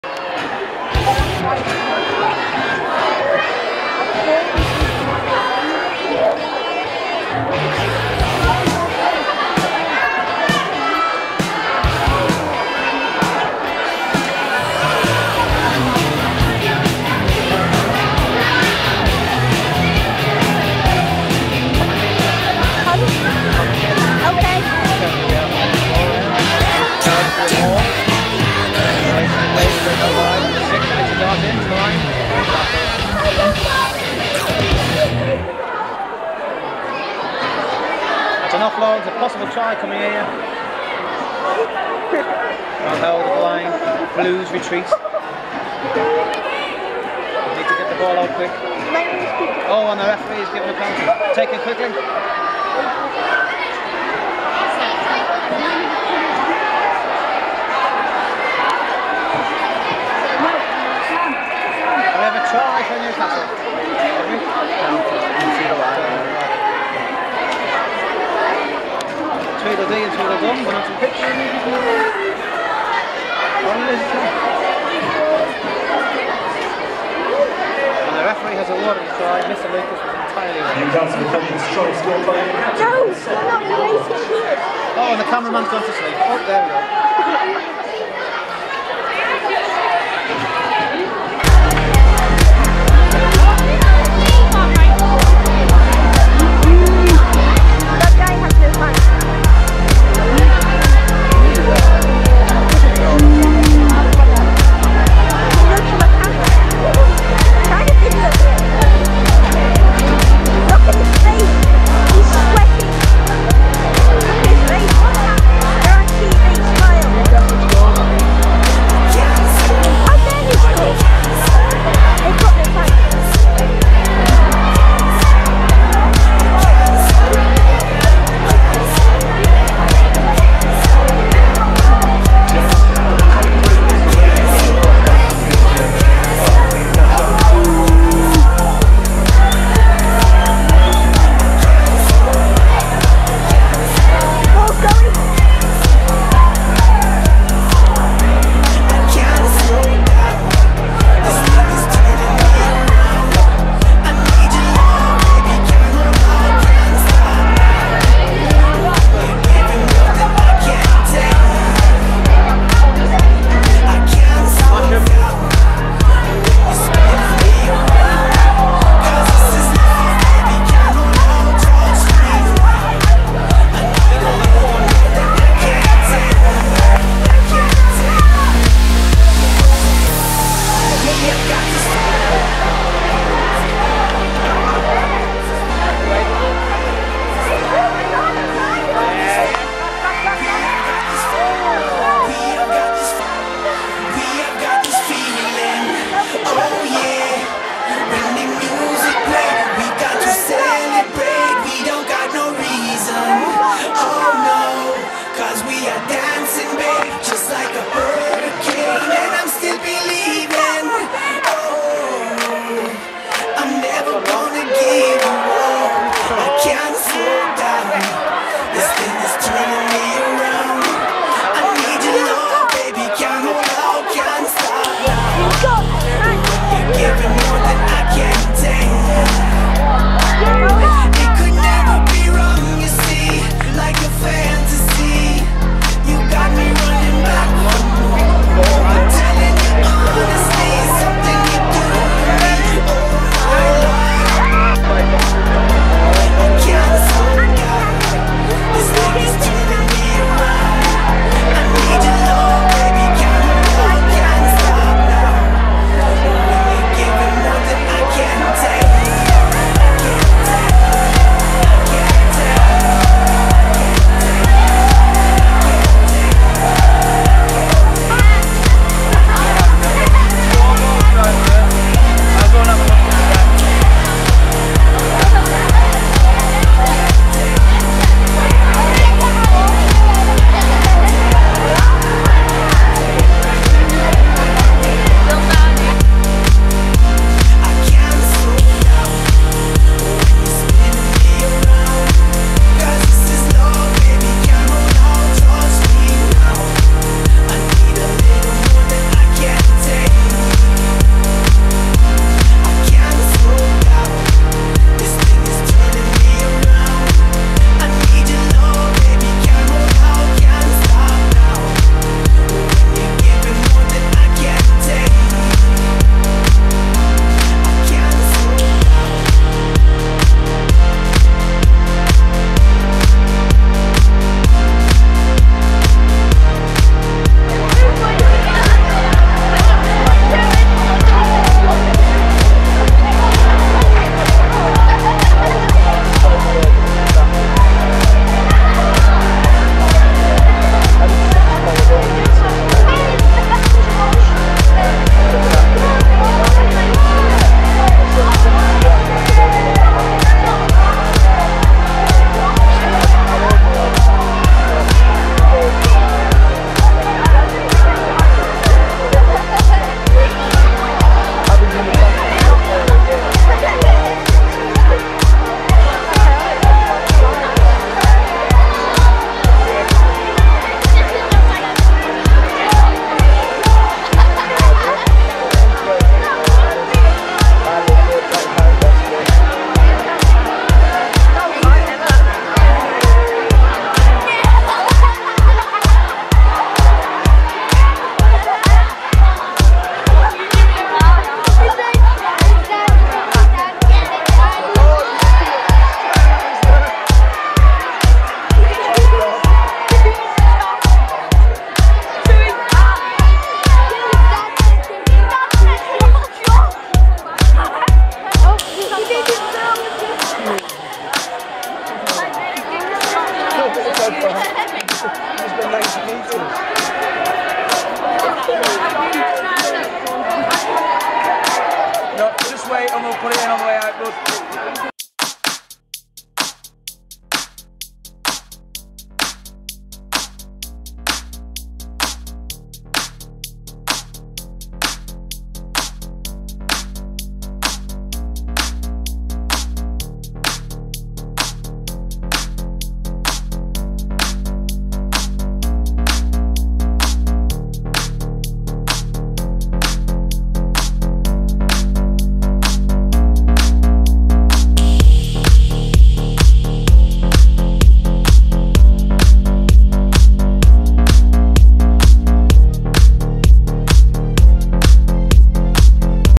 Rugby Match! (Sale Sharks VS Newcastle Falcons)
This is my video of Sale Sharks VS Newcastle Falcons! I enjoyed it very much and managed to get some great footage! I hope you enjoy this great video.